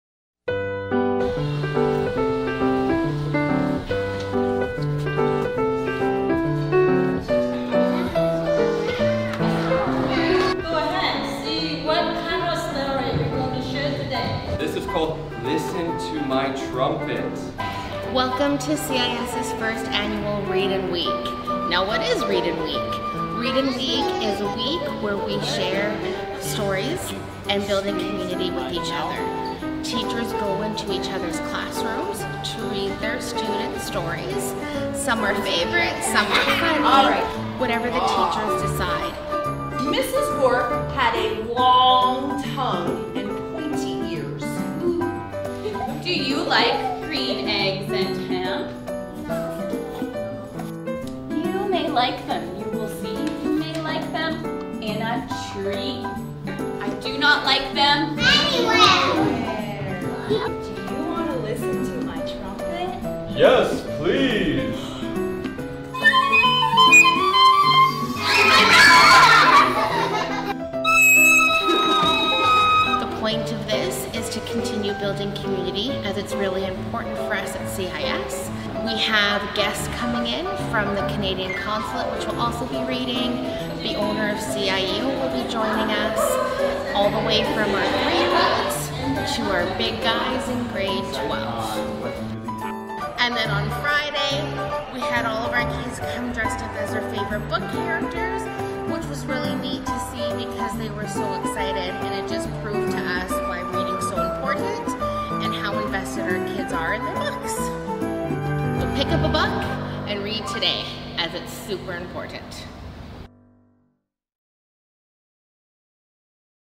Read in Week at Canadian International School of Guangzhou
Why students in Canadian International School of Guangzhou love to read?